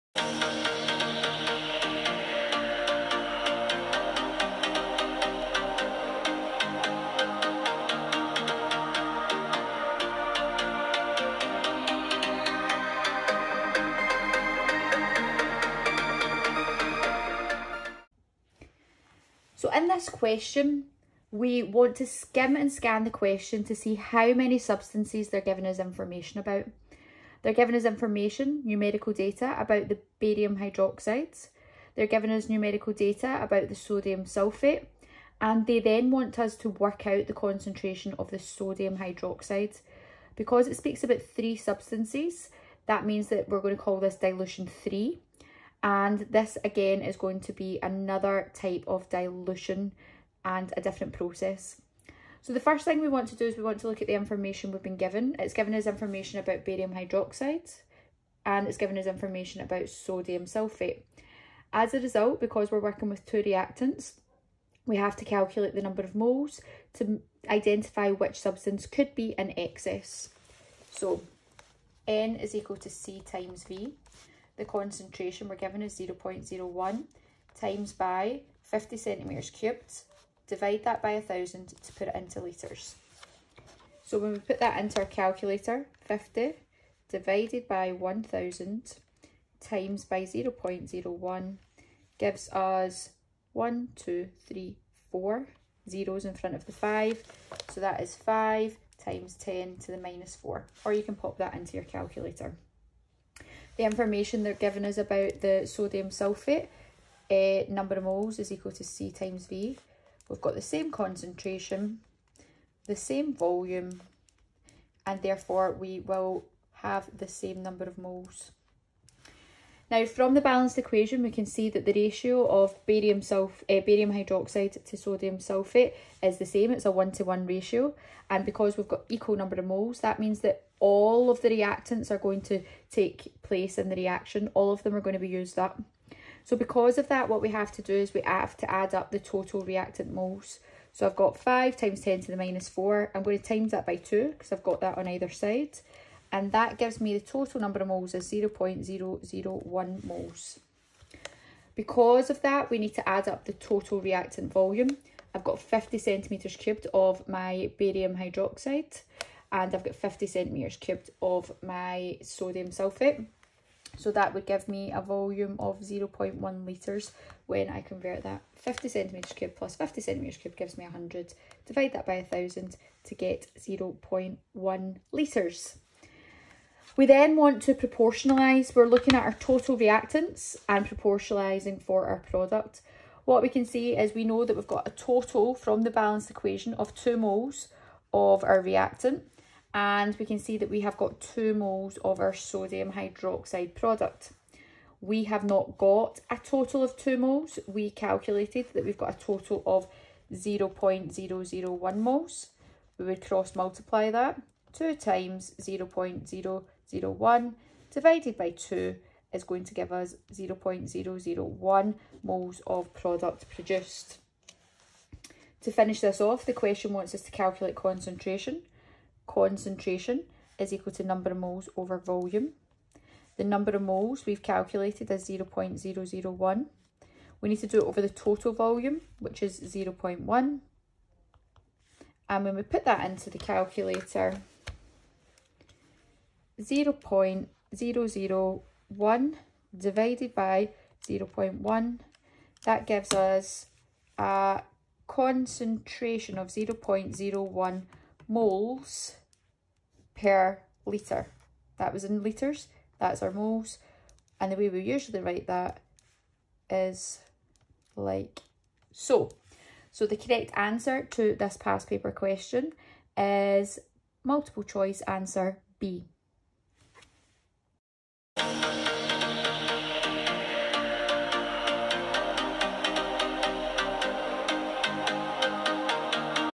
Dilution 3: How to calculate the concentration of a product solution from two reacting solutions
This calculation is known as "Dilution 3" as there are three substances we are working with in the question.

This is a step by step guide of how to calculate the concentration of a product solution from two reacting solutions

1. Calculate number of moles of each reactant
2. Add total moles of reactant and total volume of reactant
3. Mole ratio the total reactants to product
4. Substitute c = n / total volume of reactants